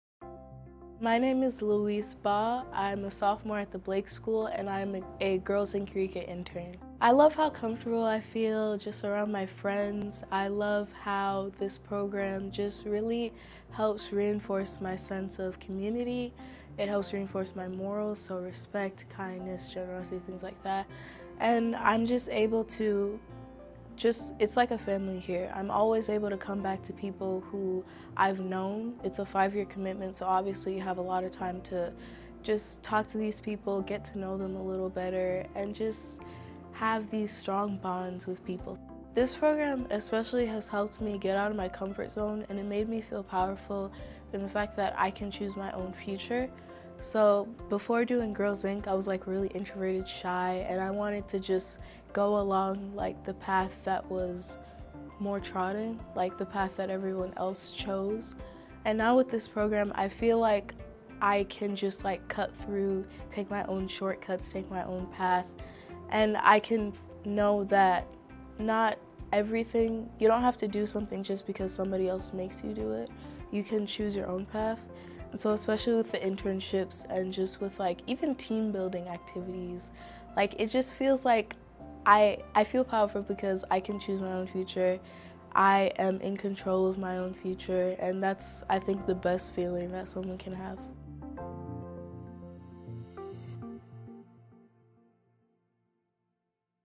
YWCA Girls Inc Program Participant Louise Feels Powerful Because...
Louise, a Girls Inc. participant at YWCA Minneapolis, shares how her experience with YWCA has impacted her life, including feeling empowered to take her own path in life.